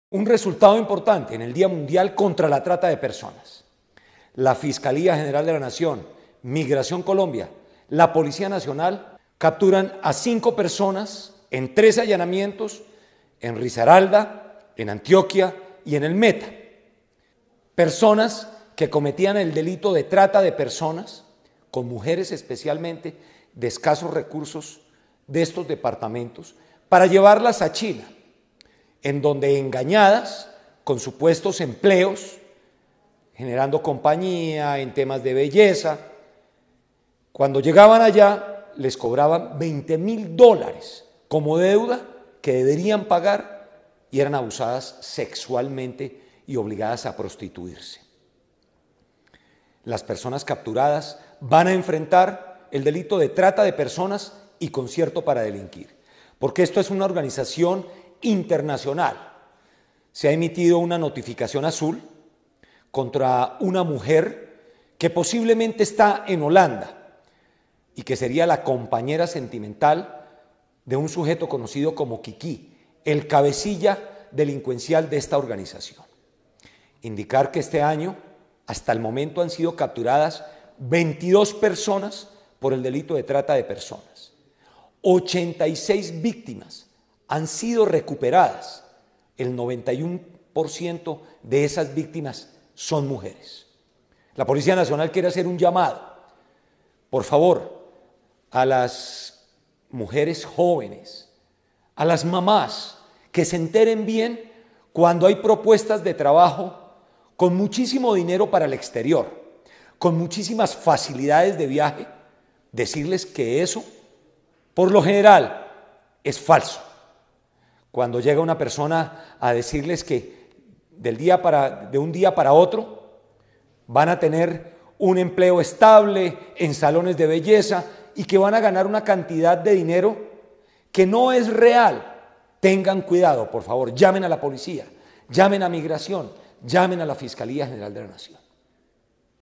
Desmantelan bandas delincuenciales contra la trata de personas.
Hoy es el DíaMundialContraLaTrataDePersonas. El director de la Policía, generam Luis Vargas, se refirió al desmantelamiento de bandas delincuenciales dedicadas a esta reprochable actividad.